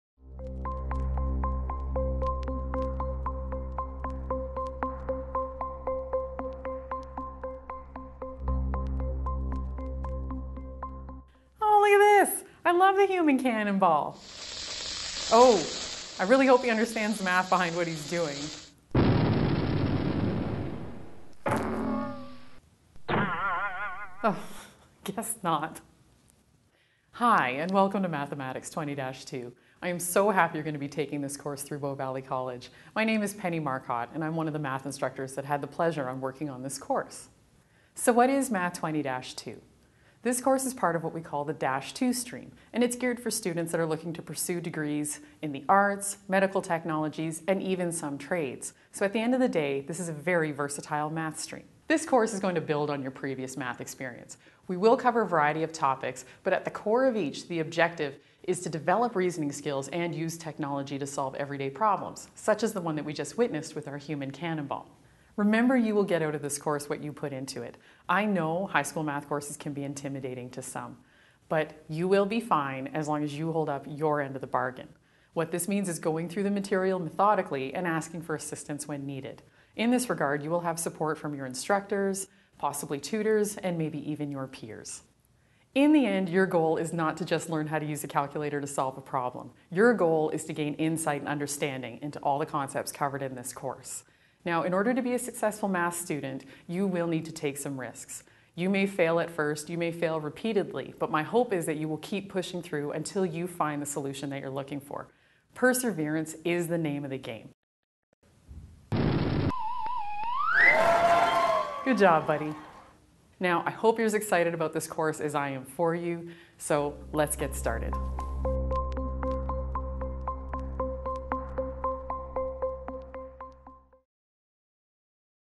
Introduction to Mathematics 20-2
This is an introductory video for the Mathematics 20-2 offered by the Centre for Excellence in Foundational Learning, Bow Valley College. The course is designed for the students who pursue degrees in the arts, medical technologies,and some trades.